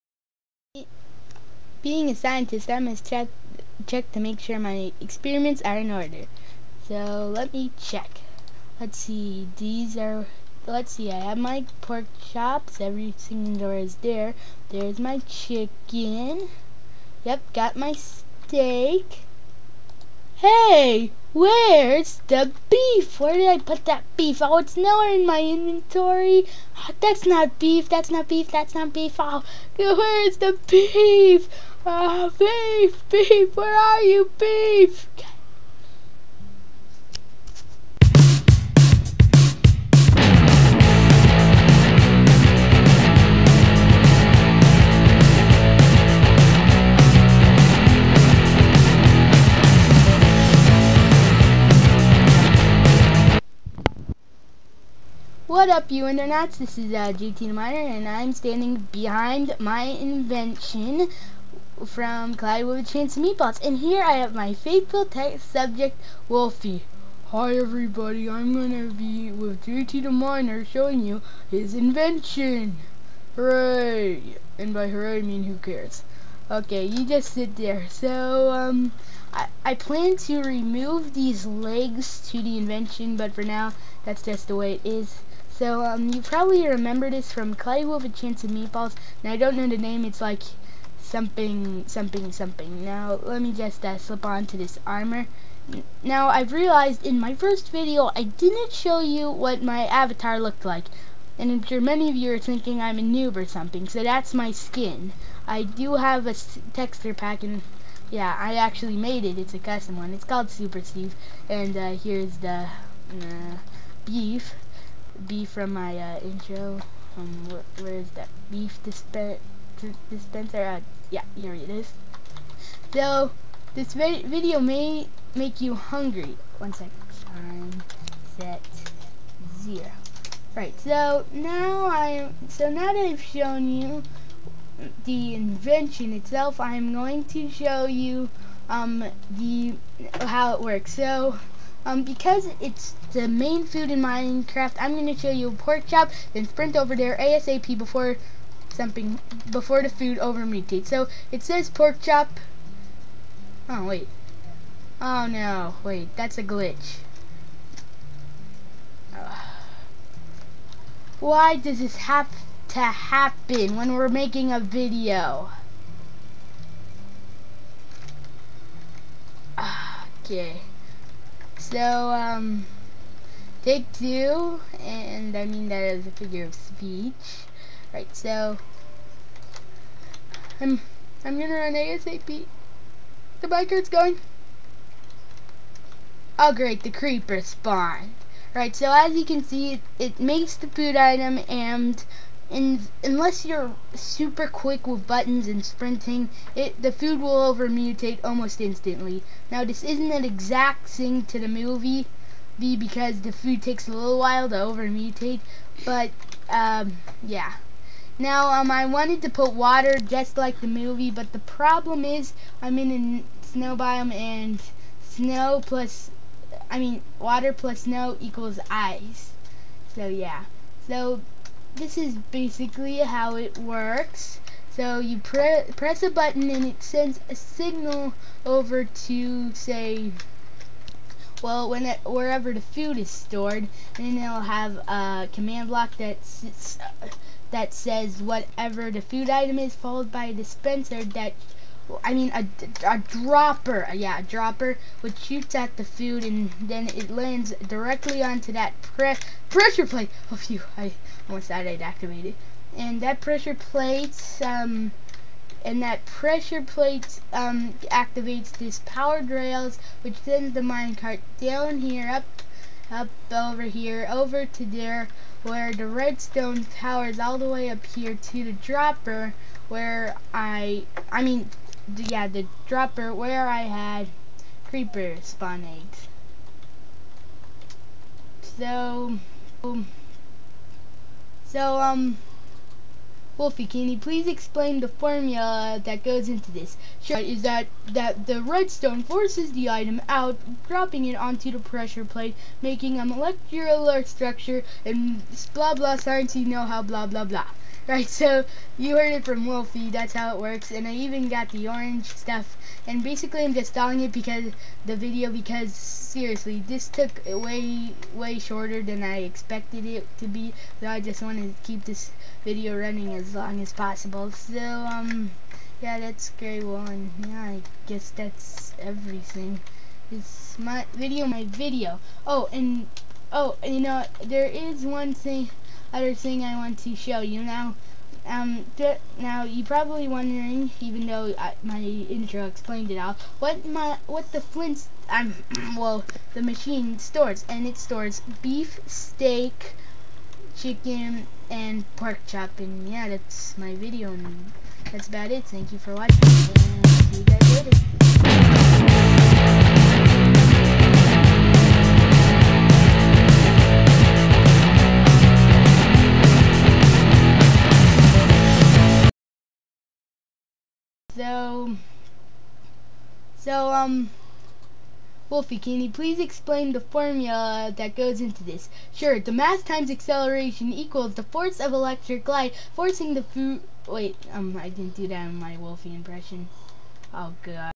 J.T. The Miner's FLDSMDFR
This is the next video in the J.T. the Miner saga.  It's about the machine in "Cloudy with a Change of Meatballs".  I hope you enjoy it and give a shout out to Wolfy.